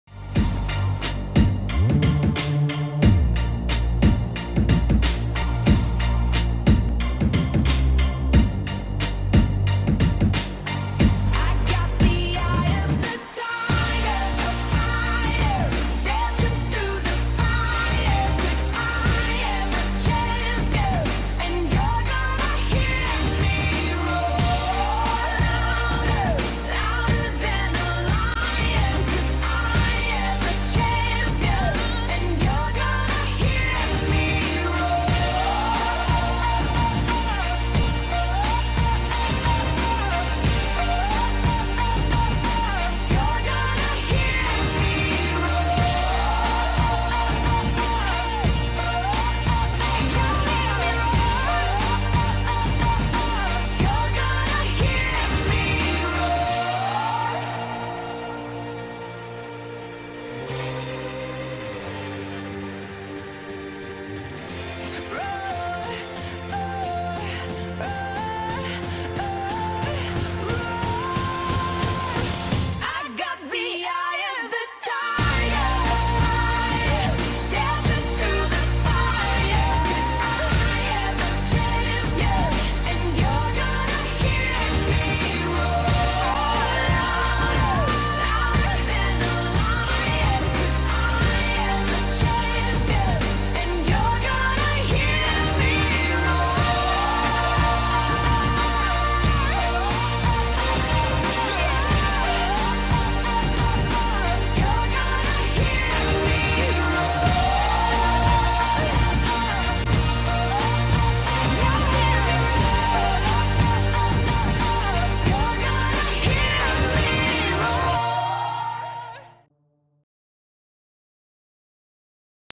UNLV Women's Basketball Offseason 2013-14
Lady Rebs working hard in preparation for the upcoming season!